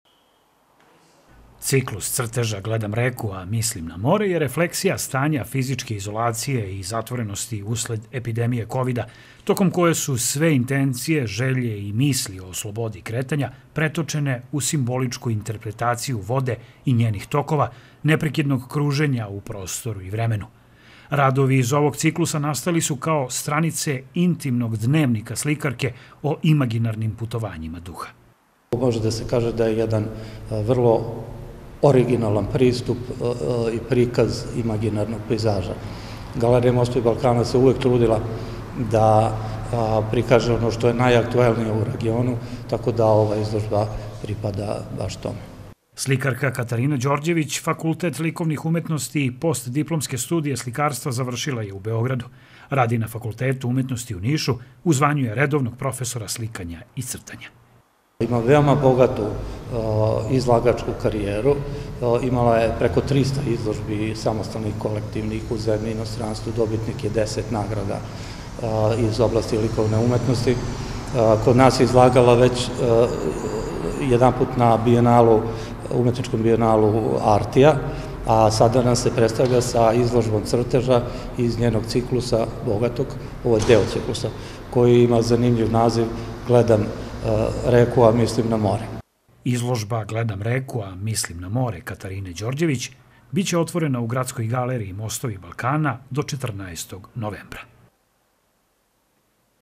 Crteži Katarine Đorđević u Gradskoj galeriji „Mostovi Balkana“
„Gledam reku, a mislim na more“ naziv je izložbe crteža Katarine Đorđević, slikarke iz Niša.